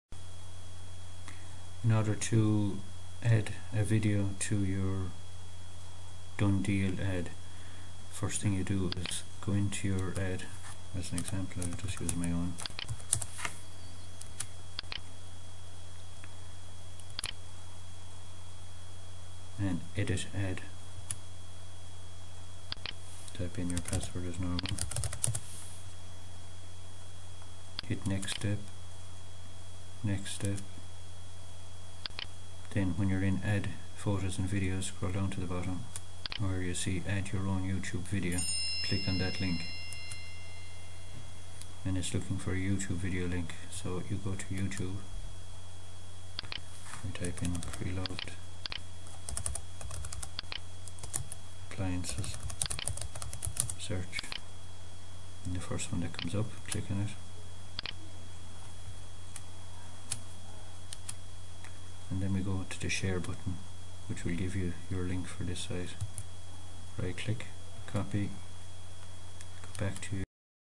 Uploading Youtube video to Donedeal Ad
How to add a Youtube video to your Donedeal Ad..